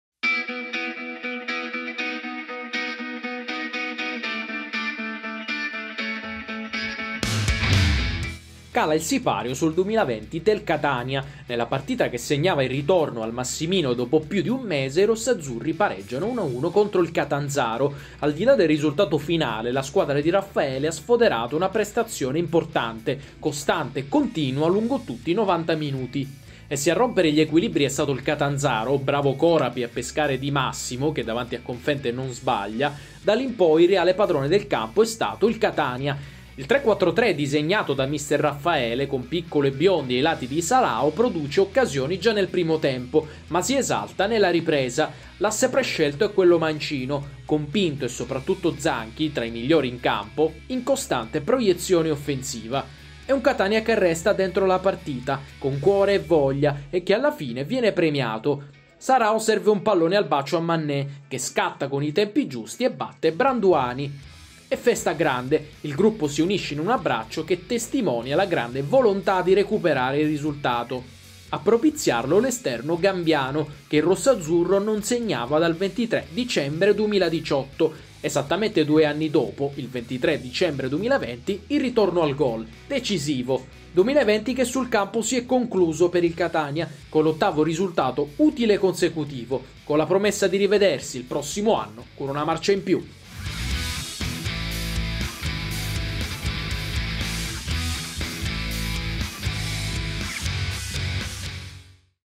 Catania 1-1 Catanzaro | Di Massimo apre, Manneh risponde: pari al Massimino | Serie C Highlights
Catania e Catanzaro pareggiano 1-1 al Massimino nella loro ultima gara del 2020. Vantaggio giallorosso con Di Massimo, pari rossazzurro di Manneh. Il servizio di Simone Vicino.